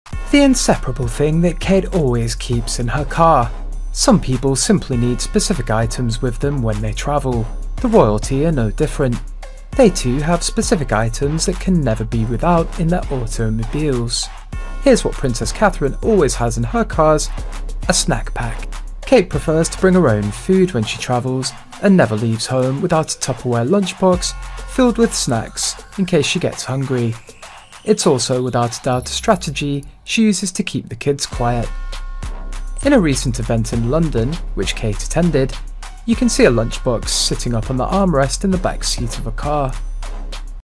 The Inseparable Thing That Kate Always Keeps In Her Car
Some people simply need specific items with them when they travel. The royalty is no different.  They too have specific items can never be without in their automobiles.